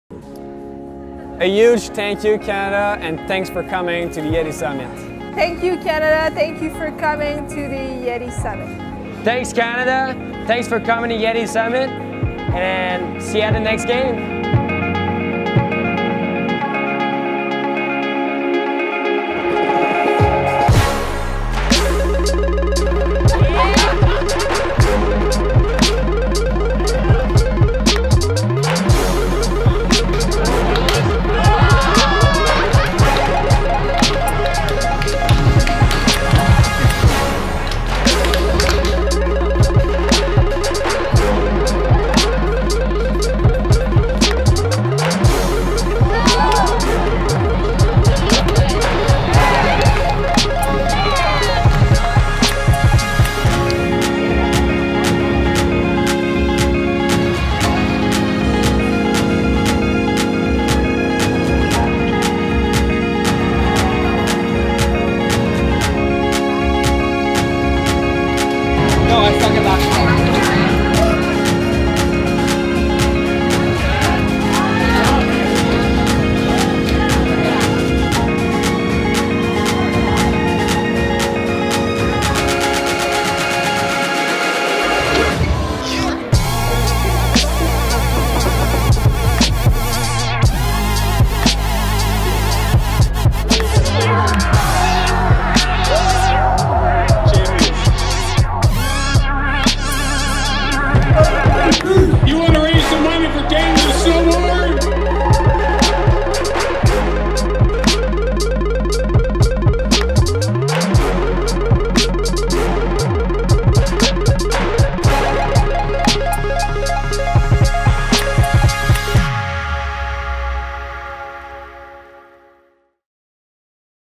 Canada Snowboard - Yeti Summit 2018 - Highlight Video
Produced for: Canada Snowboard

Project Description: Event highlight video for Canada Snowboard of their fundraising event the Yeti Summit; raising funds for the National, Paralympic and Olympic Snowboard teams. The Illustrations were done by Alexander Beebe.

Work Completed: Camera Operation, Gimbal Operation, Motion Graphics and Editing